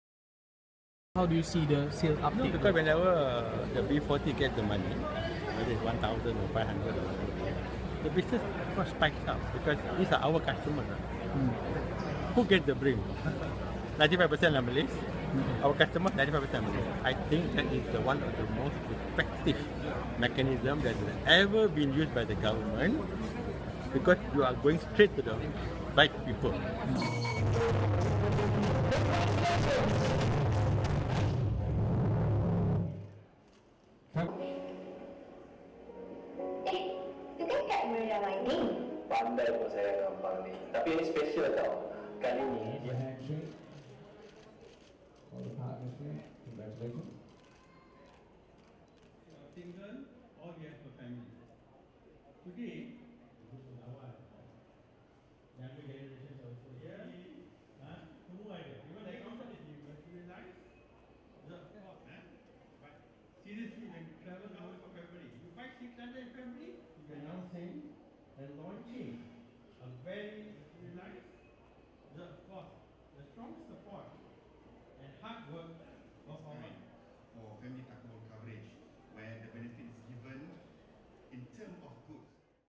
Mydin: Slow Growth In Hypermarket Next Year
Hypermarket operator, Mydin Mohamed Holdings Bhd, expects slow growth for the local retail industry next year but is hopeful that the upcoming general elections next year, will help boost consumer spending.

Its Managing Director, Datuk Ameer Ali Mydin said consumer per basket sales have been slowing down but noted the effectivenees of goverment initiative such as BR1M, to help the B40 group.

He was speaking after the launch of iMeriah Takaful coverage plan -- a collaboration between AmMetLife Takaful and Mydin, available exclusively for eligible Mydin Meriah card members, nationwide.

The takaful coverage plan is a unique and innovative shariah compliant takaful plan, which provides coverage against death with takaful benefits in the form of goods instead of cash.

(Soundbite of Datuk Ameer Ali Mydin/Managing Director, Mydin Mohamed Holdings Bhd)